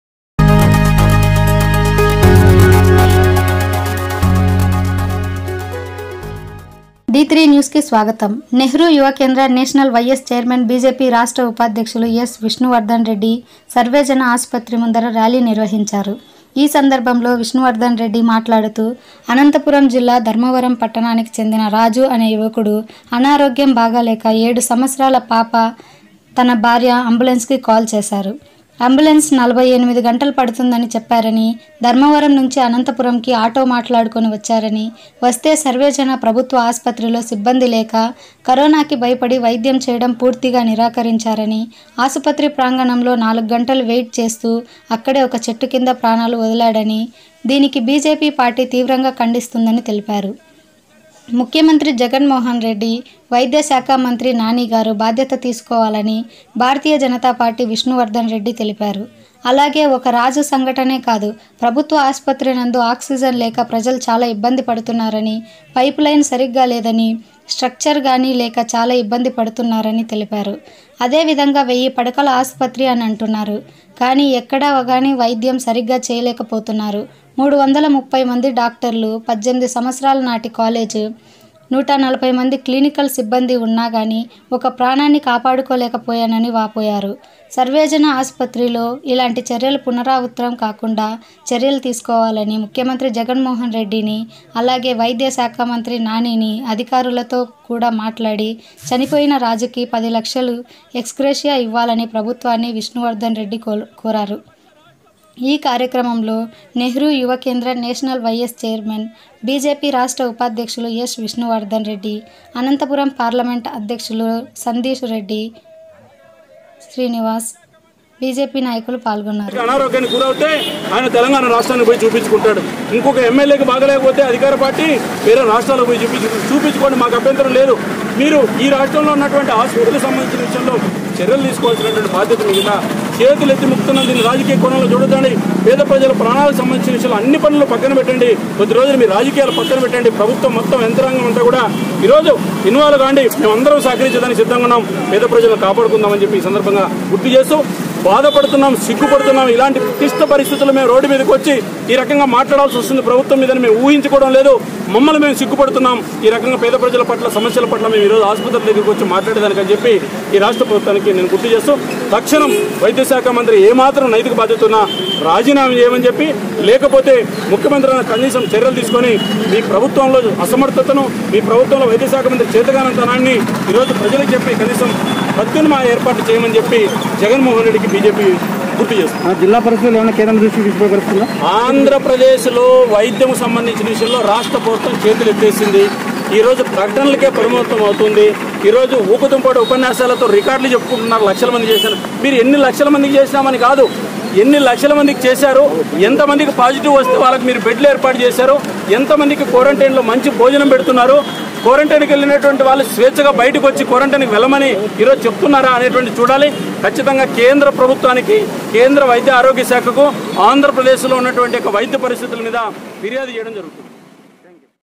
బిజెపి పార్టీ విష్ణువర్ధన్ రెడ్డి అనంతపురము సర్వజన ఆసుపత్రి ముందర నిరసన నిర్వహించారు
నెహ్రూ యువ కేంద్ర నేషనల్ వైస్ చైర్మన్ బిజెపి రాష్ట్ర ఉపాధ్యక్షులు యస్, విష్ణువర్ధన్ రెడ్డి సర్వజన ఆసుపత్రి ముందర  నిరసన నిర్వహించారు. ఈ సందర్భంలో విష్ణువర్ధన్ రెడ్డి మాట్లాడుతూ..
అనంతపురం జిల్లా ధర్మవరం పట్టణానికి చెందిన రాజు అనే యువకుడు అనారోగ్యం   బాగా లేక ఏడు సంవత్సరాల పాప తన భార్య  అంబులెన్స్ కి కాల్ చేశారు.అంబులెన్స్  48 గంటలు పడుతుందని చెప్పారనీ,ధర్మవరం నుంచి అనంతపురం కు ఆటో మాట్లాడుకుని వచ్చారని,వస్తే సర్వజన ప్రభుత్వ ఆసుపత్రిలో సిబ్బంది లేక  కరోనా కి భయపడి  వైద్యం చేయడం పూర్తిగా నిరాకరిం చారని.ఆసుపత్రి ప్రాంగణంలో నాలుగు గంటలు వెయిట్ చేస్తూ అక్కడే ఒక చెట్టు కింద ప్రాణాలు వదిలా డని. దీనికి బిజెపి పార్టీ తీవ్రంగా ఖండిస్తోందని తెలిపారు.  ముఖ్యమంత్రి జగన్ మోహన్ రెడ్డి , వైద్య శాఖ మంత్రి నాని గారు బాధ్యత తీసుకోవాలని, భారతీయ జనతా పార్టీ విష్ణువర్ధన్ రెడ్డి తెలిపారు. అలాగే  ఒక రాజు సంఘటనే కాదు ప్రభుత్వ ఆసుపత్రి నందు ఆక్సిజన్ లేక ప్రజలు చాలా ఇబ్బంది  పడుతున్నారని, పైపులైను సరిగ్గా లేదనీ,స్ట్రక్చర్ గాని లేక చాలా ఇబ్బంది పడుతున్నారని తెలిపారు.అదేవిధంగావెయ్యి పడకల ఆసుపత్రి అని అంటున్నారు గానీ ఎక్కడ గాని వైద్యం సరిగా చేయలేక పోతున్నారు. 330 మంది డాక్టర్లు  18 సంవత్సరాల నాటి కాలేజీ,  140 మంది క్లినికల్ సిబ్బంది ఉన్నాగాని ఒక ప్రాణాన్ని కాపాడలేకపోయారని వాపోయారు.  సర్వజన ఆస్పత్రి లో ఇలాంటి చర్యలు పునరావృతం కాకుండా చర్యలు తీసుకోవాలని ముఖ్యమంత్రి జగన్ మోహన్ రెడ్డి ని అలాగే వైద్య శాఖ మంత్రి నాని ని అధికారులు  తో కూడా మాట్లాడి చనిపోయిన రాజుకి 10 లక్షలు ఎక్స్గ్రేషియా ఇవ్వాలని ప్రభుత్వాన్ని విష్ణువర్ధన్ రెడ్డి కోరారు. ఈ కార్యక్రమంలో నెహ్రూ యువ కేంద్ర నేషనల్ వైస్ చైర్మన్ బిజెపి రాష్ట్ర ఉపాధ్యక్షులు యస్. విష్ణువర్ధన్ రెడ్డి ,  అనంతపురం  పార్లమెంట్ అధ్యక్షులు సందిరెడ్డి శ్రీనివాస్  , బిజెపి నాయకులు పాల్గొన్నారు.